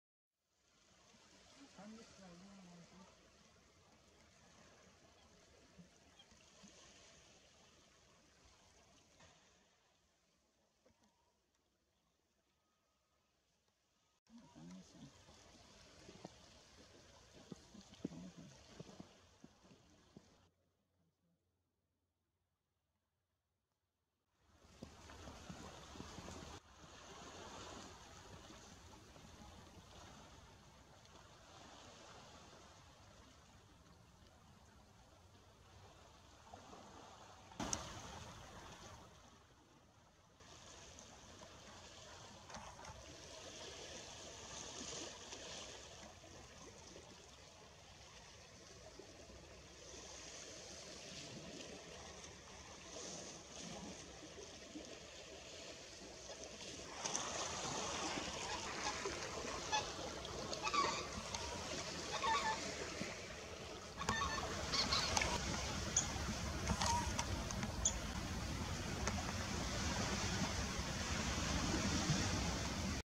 Greater White-fronted Geese in Toronto
Two Greater White-fronted Geese spent several days feeding on grass emerging through melting snow at Bluffers Park in Toronto. They also fed on algae-covered rocks. Unable to climb over ice-coated rocks, they flew, vocalizing as they went on shore to feed. 23 January 2020.